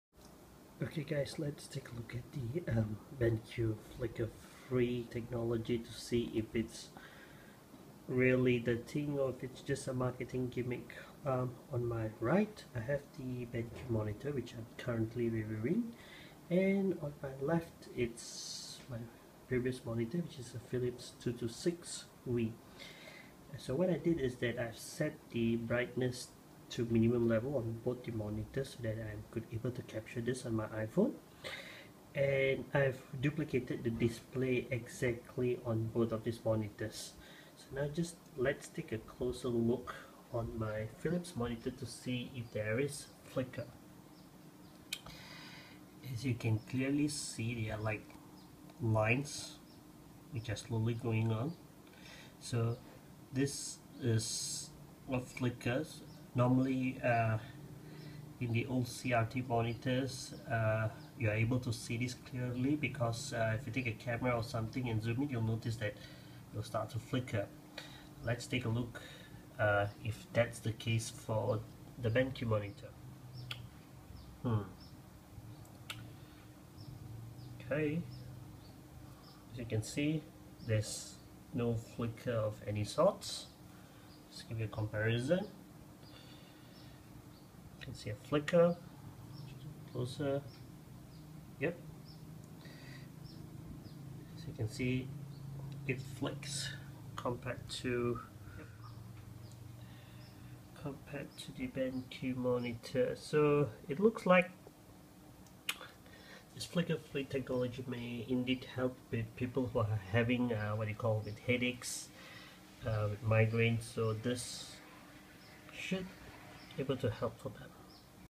A Closer Look at BenQ EW2440 Flicker-Free Technology
I'll be taking a closer look at BenQ EW2440. BenQ claims that their monitors are flicker-free. Time to put the theory to the test and if indeed what BenQ says is true.

If you have any questions,  feel free to post it here and I'll answer it

Thanks